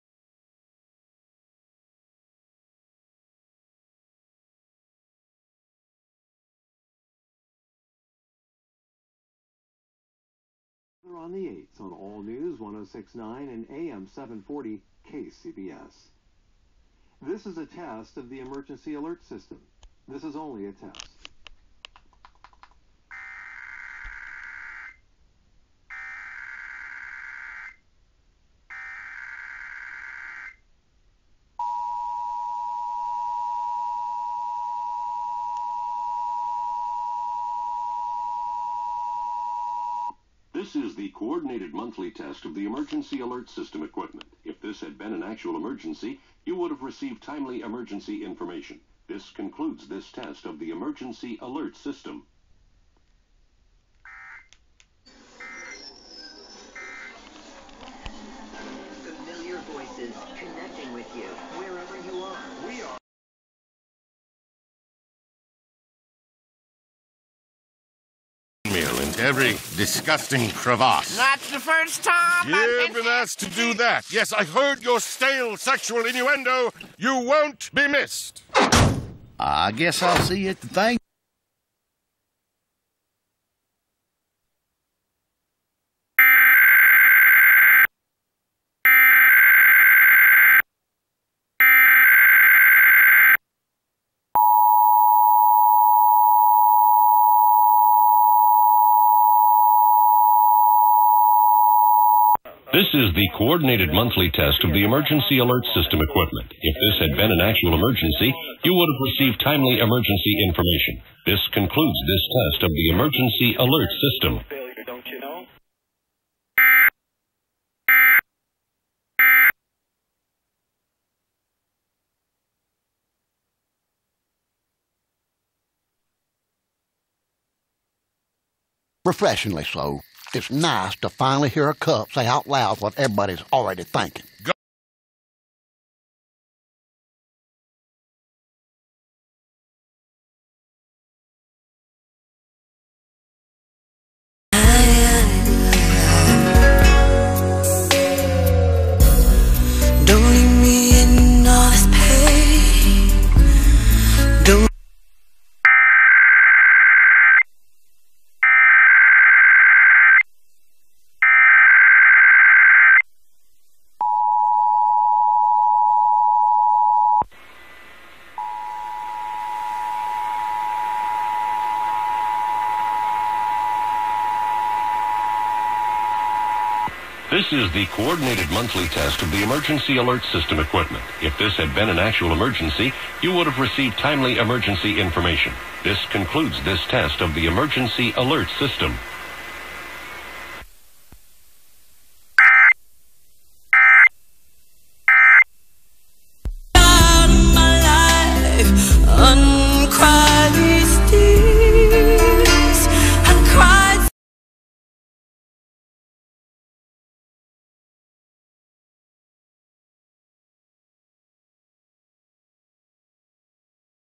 EAS Required Monthly Tests 4/14/20
This was supposed to be uploaded yesterday, but YouTube doesn't support the DV codec. So I had to use Handbrake to make it Youtube-friendly. Anyways, here is the Required Monthly Test for the SF Bay Area recorded yesterday at 4:49 AM.

Test #1: Unknown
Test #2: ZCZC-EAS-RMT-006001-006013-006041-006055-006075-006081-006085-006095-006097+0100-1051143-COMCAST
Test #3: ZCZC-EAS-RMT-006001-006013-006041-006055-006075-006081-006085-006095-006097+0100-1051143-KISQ